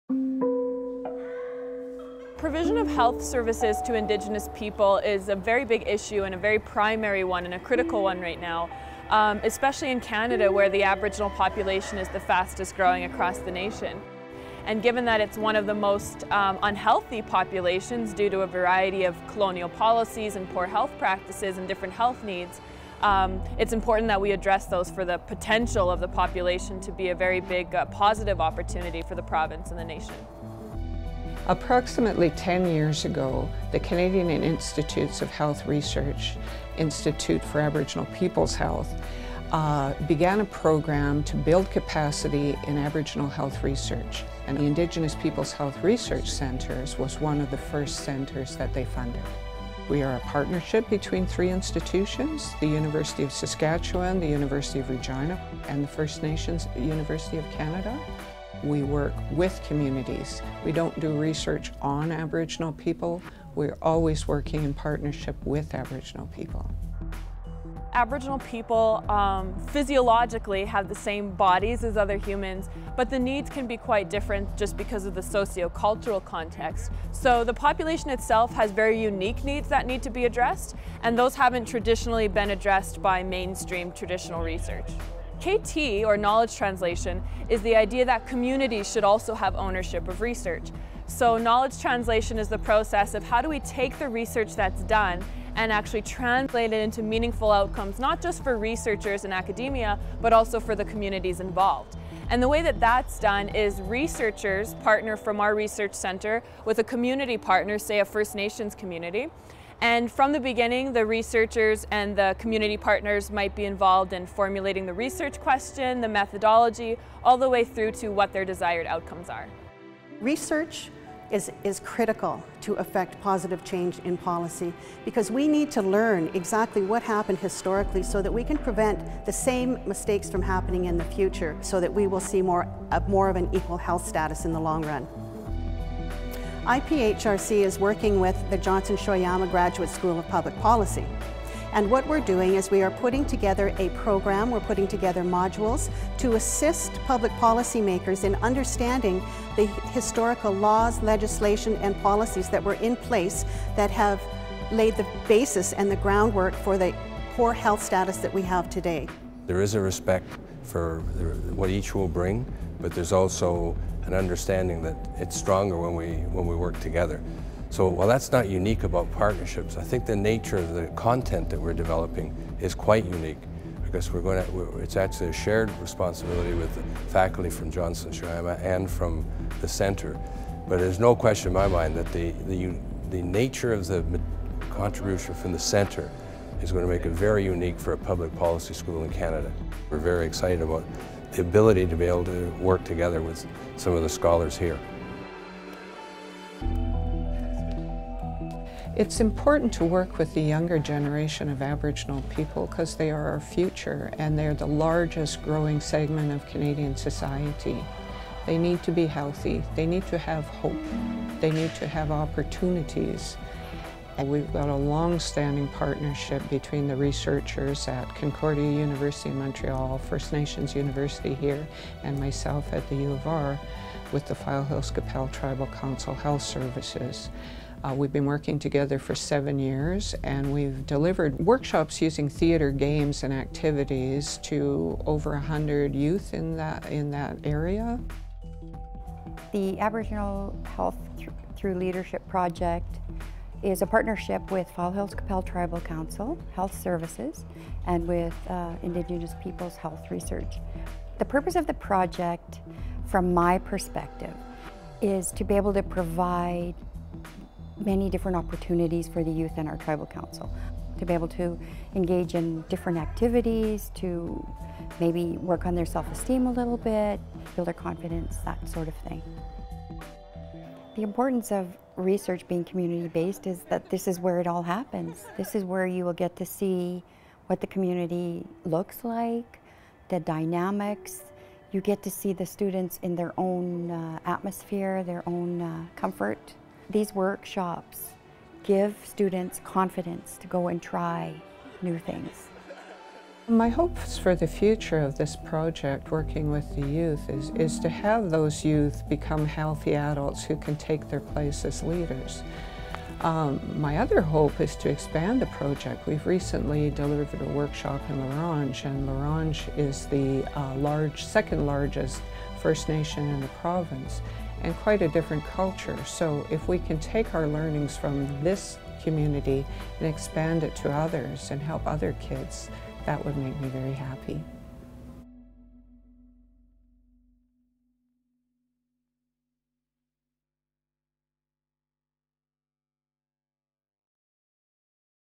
Indigenous Peoples' Health Research Centre
IPHRC is a collaborative network working to improve and strengthen the quality of Indigenous health research and the health of Indigenous people. Excellence in knowledge translation guides distinguishes IPHRC's work.